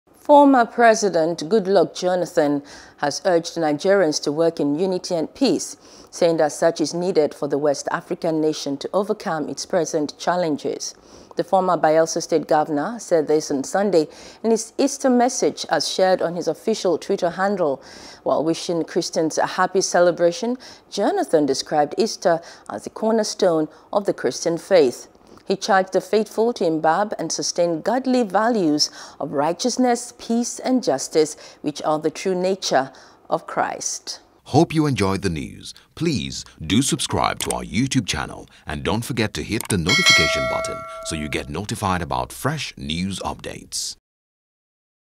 Jonathan Urges Nigerians To Work In Unity To Overcome National Challenges | NEWS
Former President Goodluck Jonathan has urged Nigerians to work in unity and peace, saying that such is needed for the West African nation to overcome its present challenges.